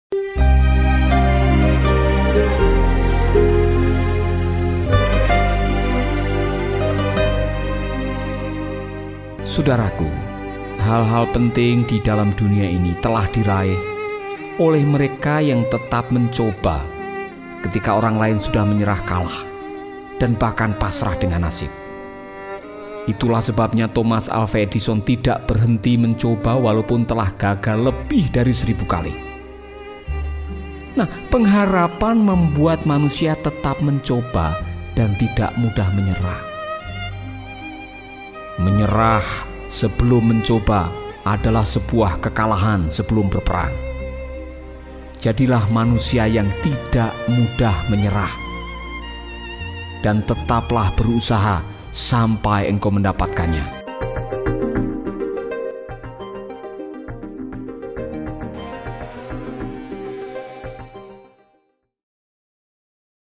Jangan Mudah Menyerah
Pengharapan membuat manusia tetap mencoba dan tidak mudah menyerah. tetaplah berusaha sampai kita mendapatkan apa yang sudah kita kejar.

sahabat juga bisa download aplikasi Making Life Better di play store atau apps store
apps         : Making Life Better